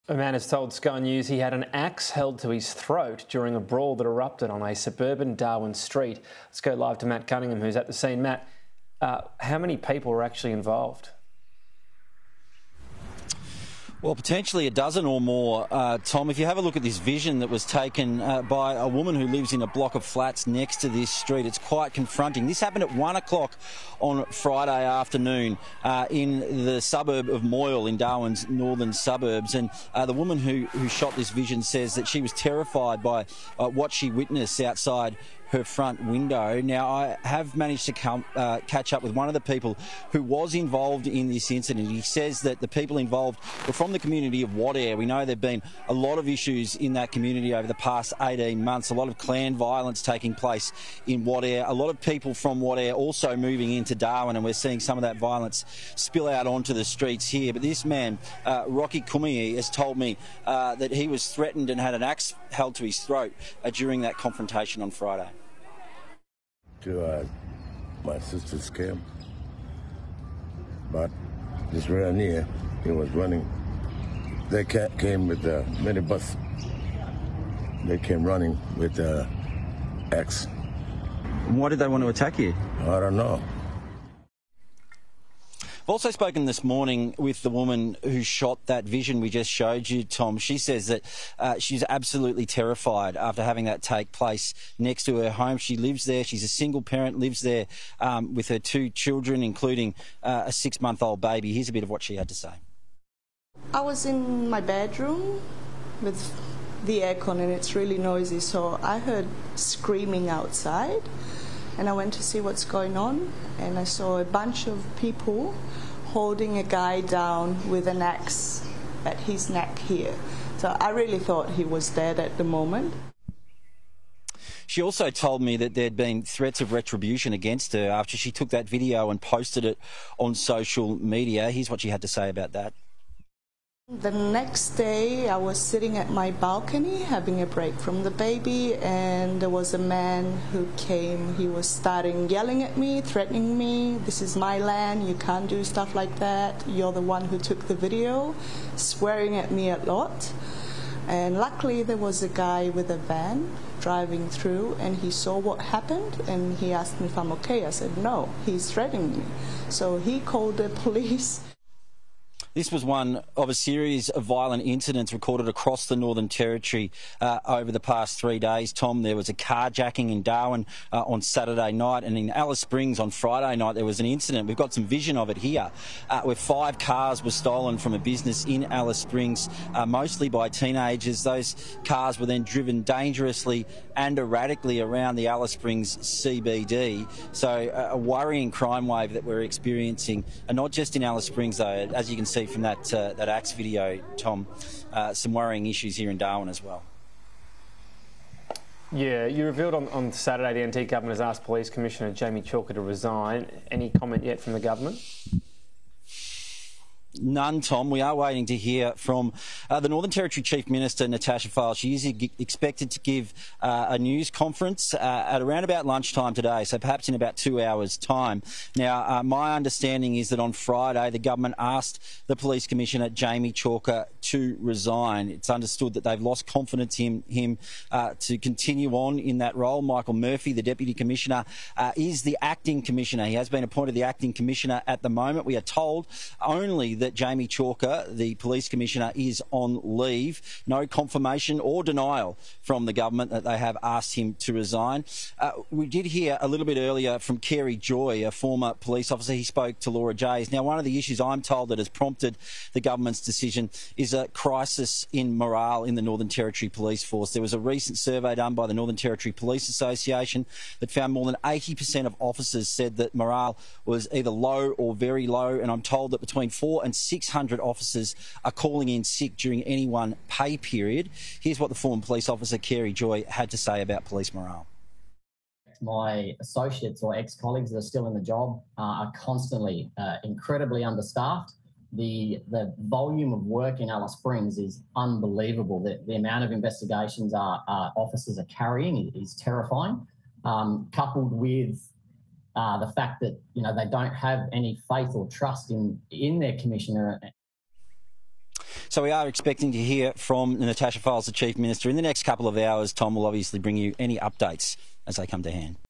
WATCH: Shocking daytime axe fight caught on camera
A man has told Sky News Australia he had an axe held to his throat during a shocking daytime brawl that erupted on a suburban Darwin street.

Potentially a dozen or more people were involved in the confronting event which was recorded from a nearby block of units.

The axe fight happened around 1pm on Friday the 31st of March in the suburb of Moil.

Victim Rocky Kumaiyi spoke to Sky News Australia about the attack, saying he doesn't know why he was targeted.

This daytime attack comes amid escalating crime and violence sweeping across the Northern Territory.